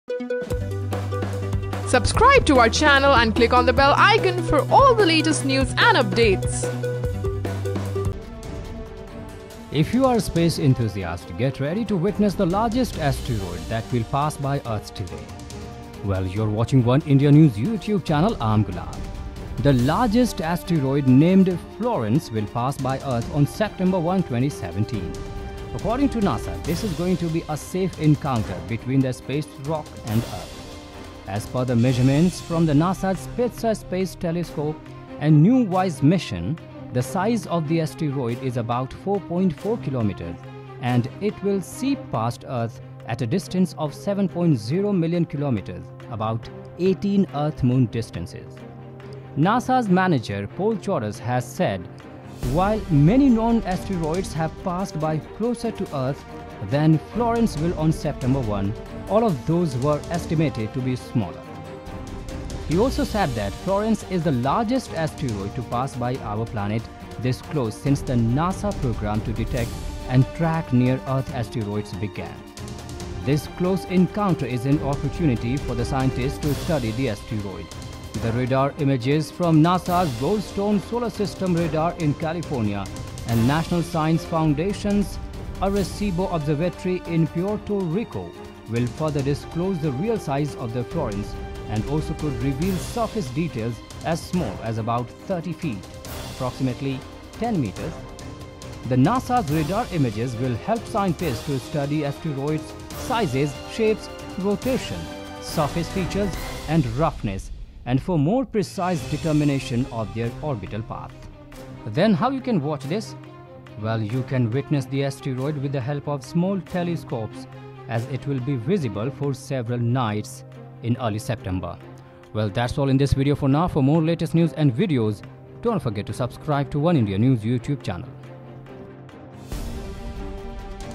NASA: Largest asteroid ‘Florence’ will pass by Earth on September 1 | Oneindia News
According to NASA, the largest asteroid named ‘Florence’ will pass by Earth on September 1, 2017. This is going to be a safe encounter between the space rock and Earth. The size of the asteroid is about 4.4 kilometres and it will seep past earth at a distance of 7.0 million kilometres (about 18 Earth-Moon distances). The space enthusiast can witness the asteroid with the help of small telescopes as it will visible for several nights between in the early September.